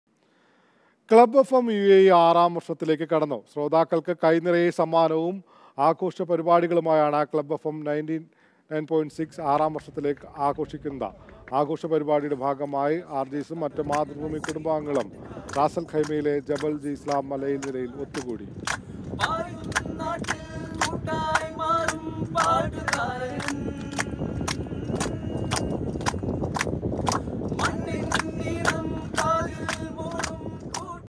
ക്ലബ്ബ് എഫ്എം യുഎഇ ആറാം വര്‍ഷത്തിലേക്ക് കടന്നു | Club FM | Mathrubhumi News
‌ശ്രോതാക്കള്‍ക്ക് കൈനിറയെ സമ്മാനവും  ആഗോഷപരിപാടികളുമായാണ് ക്ലബ്ബ് എഫ്എം 99.6 ആറാം വാര്‍ഷികം ആഘോഷിച്ചത്.

Happy viewing!